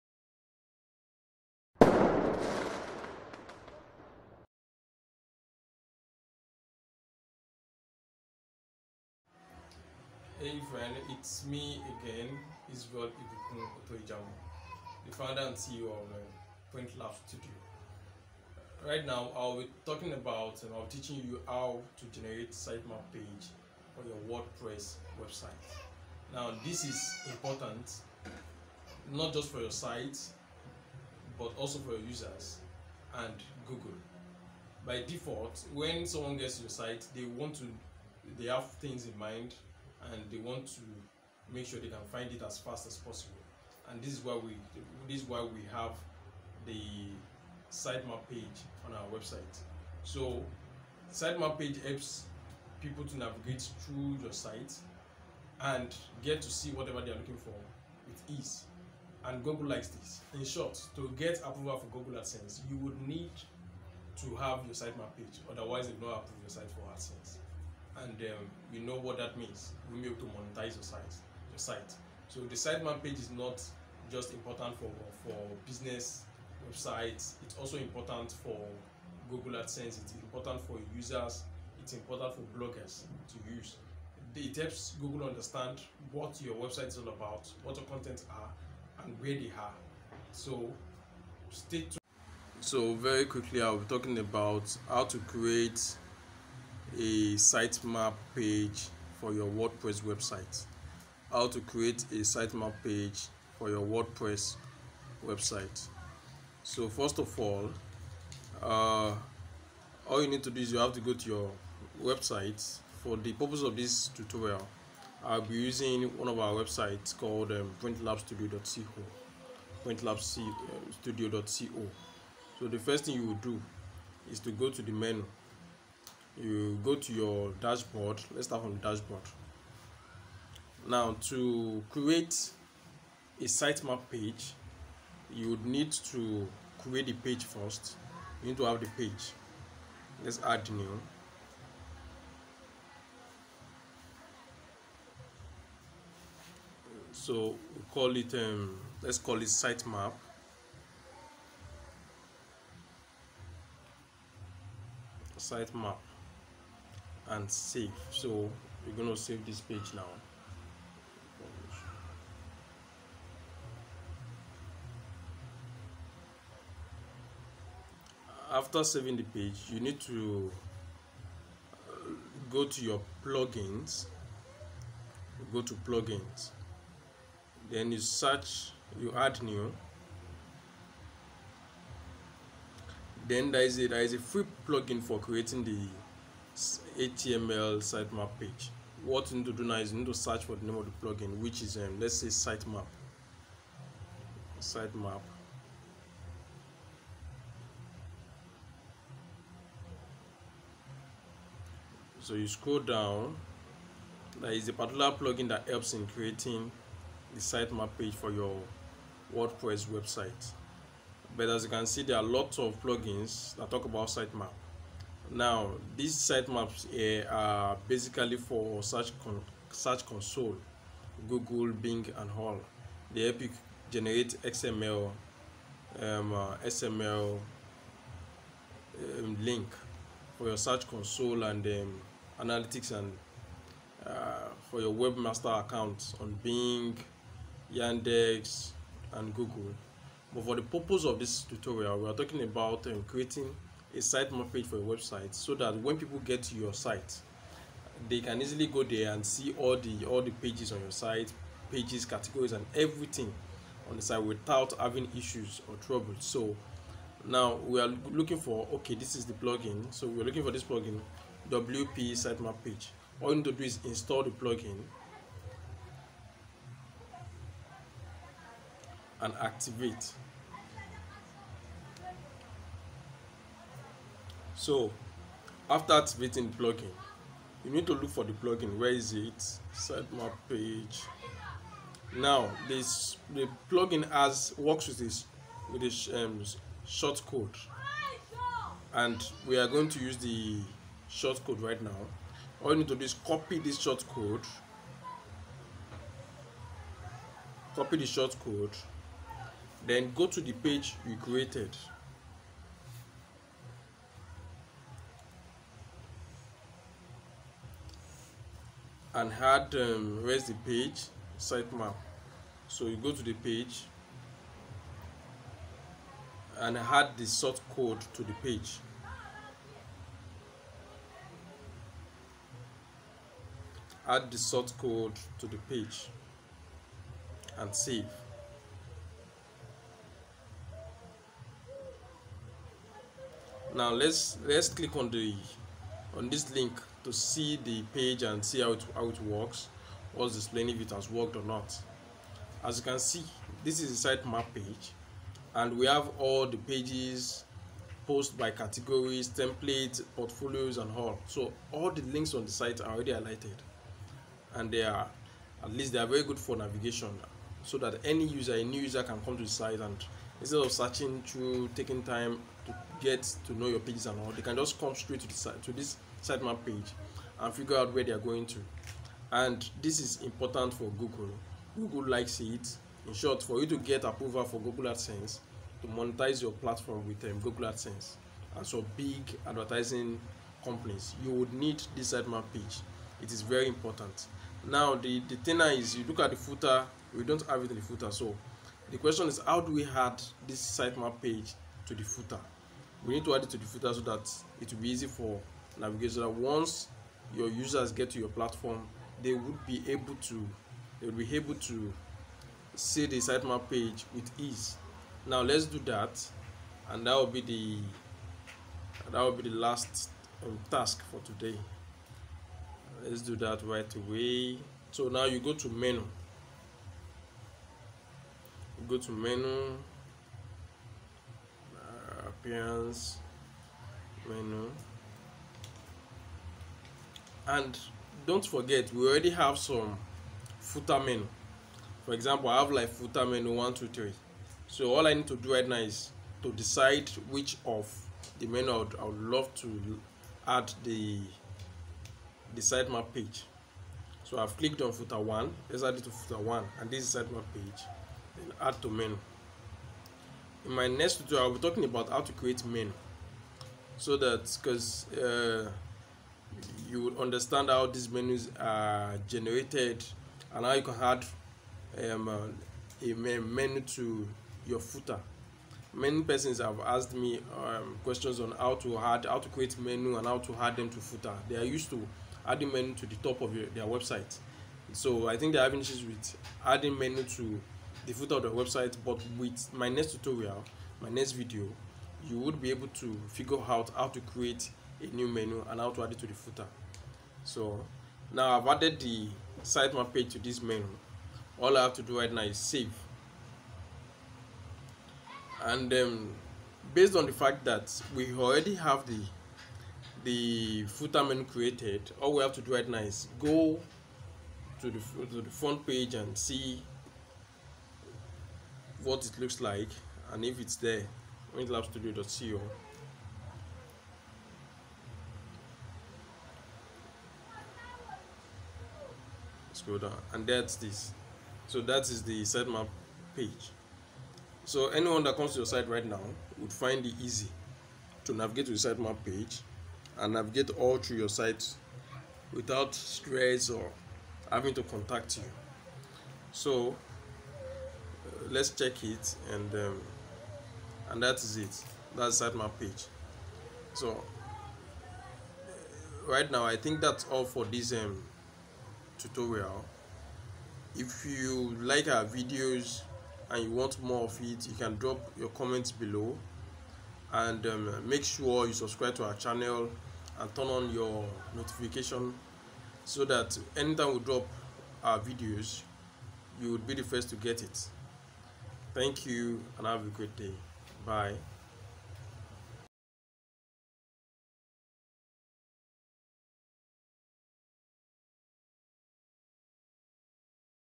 How to Create a Sitemap Page for WordPress websites.
Ever wanted to display a visual sitemap in WordPress? A visual sitemap allows you to present a hierarchical layout of your different pages and sections. In this article, we will show you how to create a visual sitemap in WordPress without writing any code.
Sitemap Page is important for user navigation and search engine bots. It helps people in navigating your website with ease, thereby achieving their goal in a short time.
Also, sitemap page helps in getting Google adsense approval fast as it is one of their requirements.